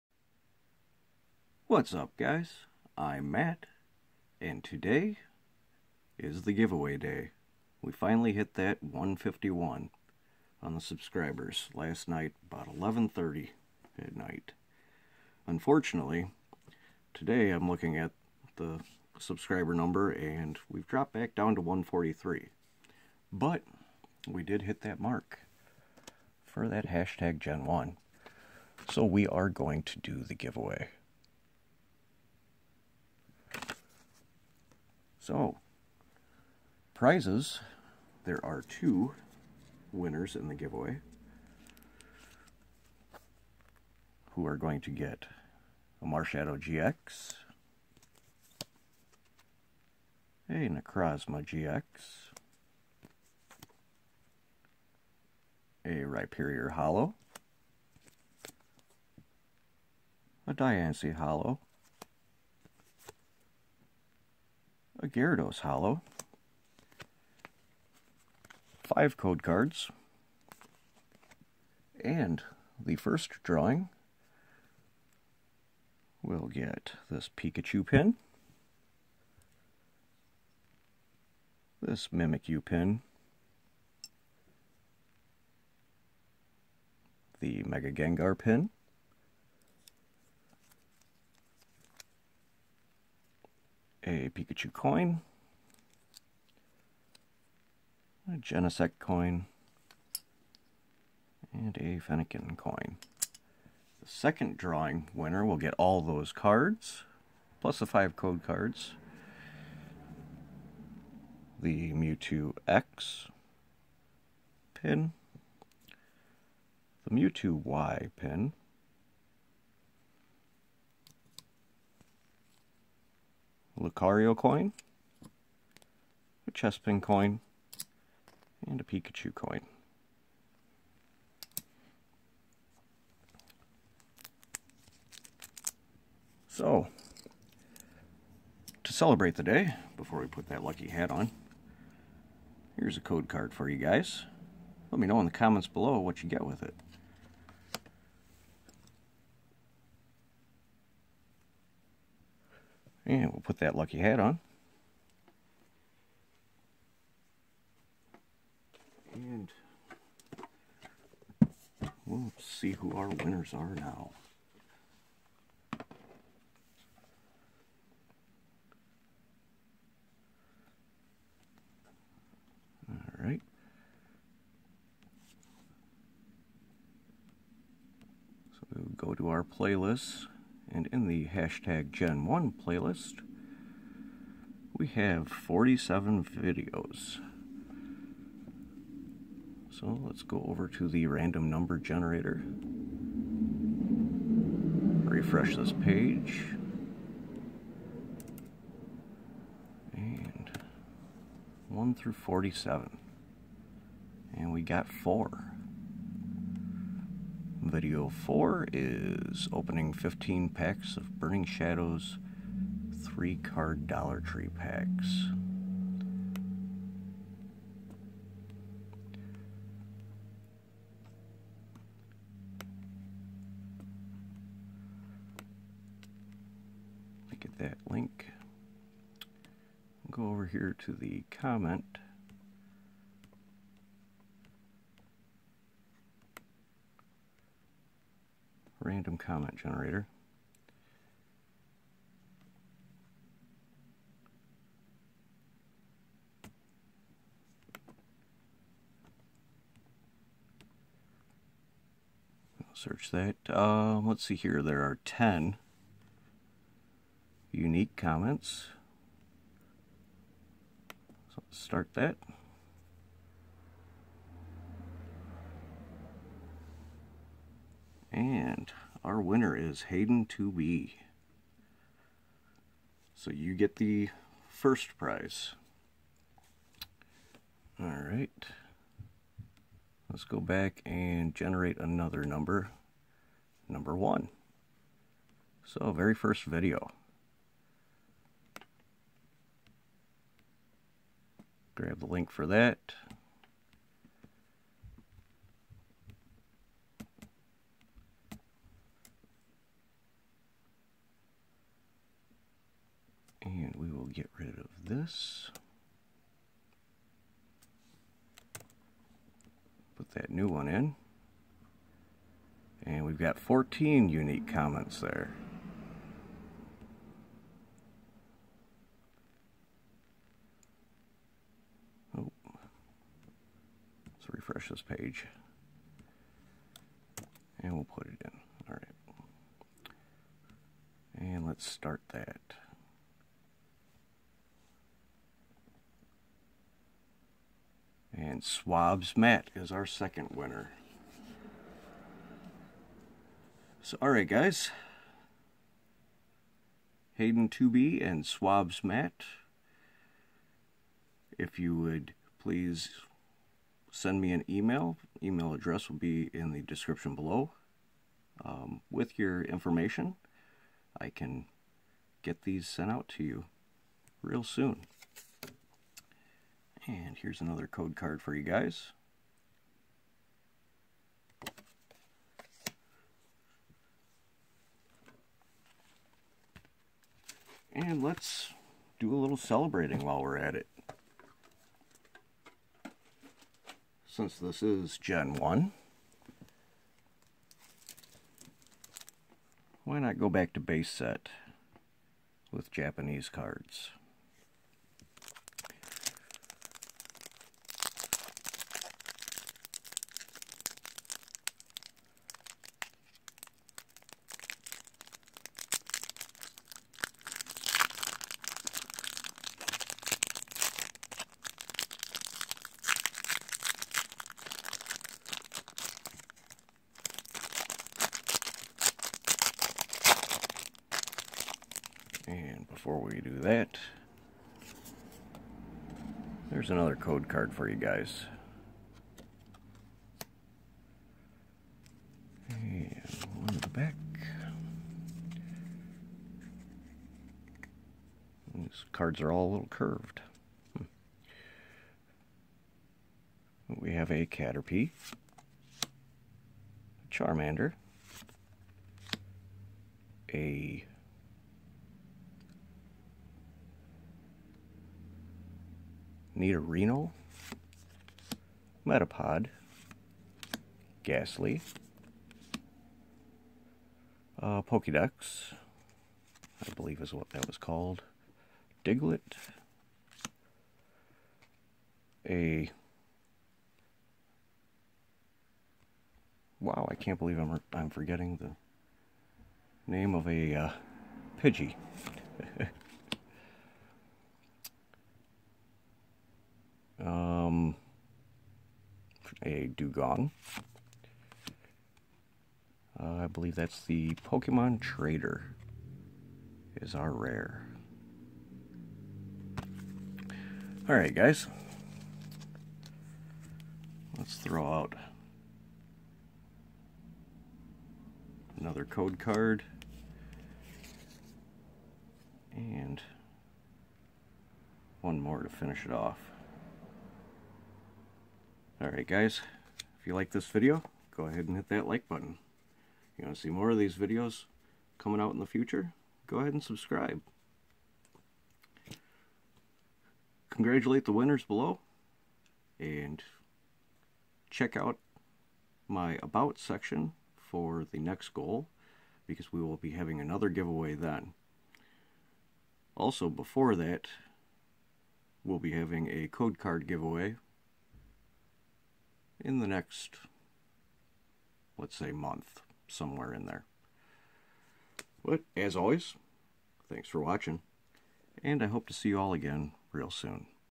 GEN1 151 Subscriber Giveaway!!!
What's up guys! Let's see who our 2 winners are, and take a look at what they are getting!